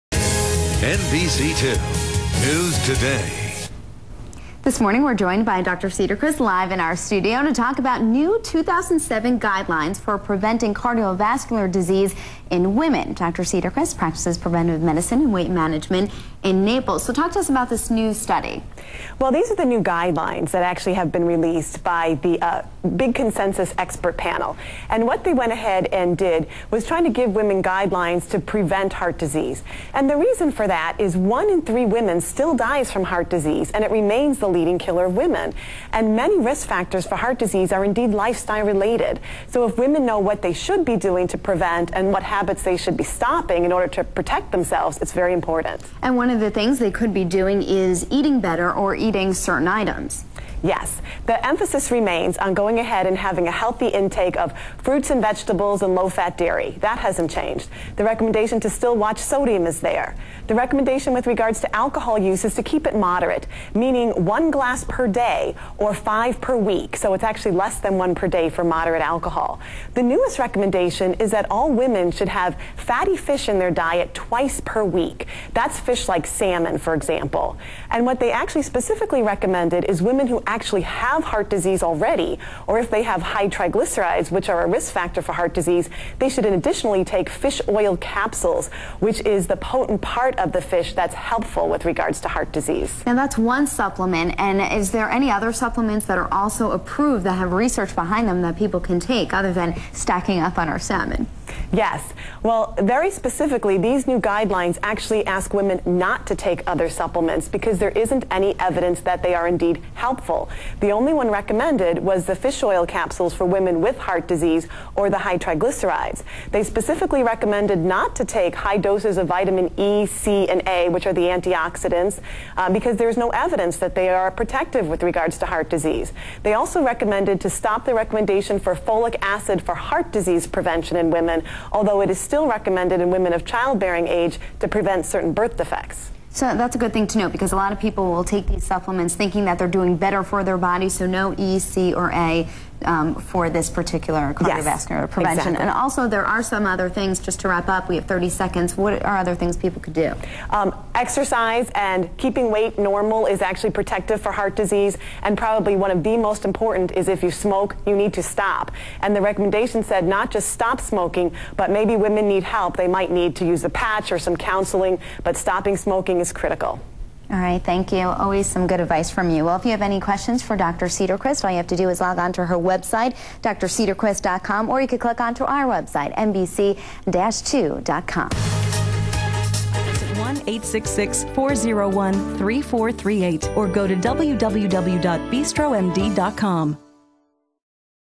Cardiovascular Disease in Women.
Dr. Cederquist reviews 2007 Guidelines for preventing Cardiovascular Disease in women.